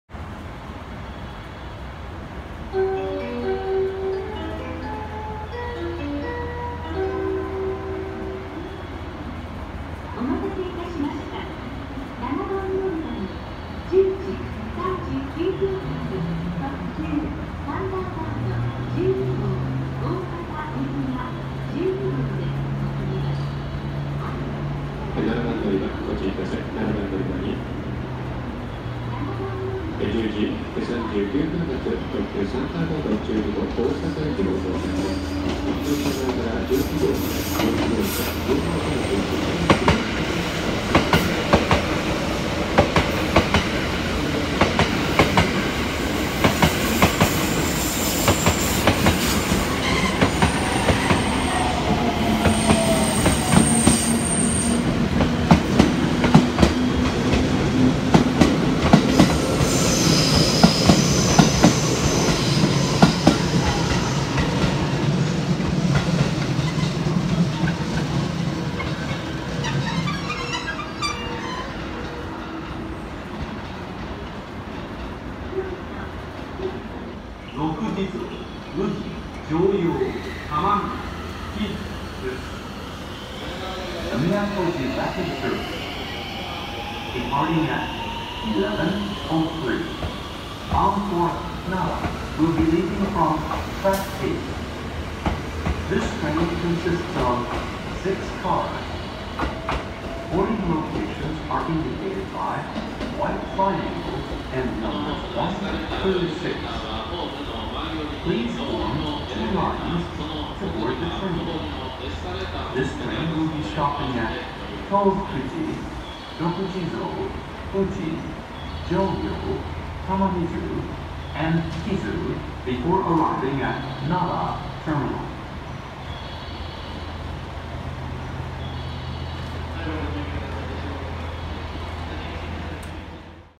12両編成！特急サンダーバード12号大阪行き@京都駅
個人的に、サンダーバード号がこの編成だとテンションあがります(*≧∀≦*)
※スマホで撮影と編集をしました。
(2018/3/3)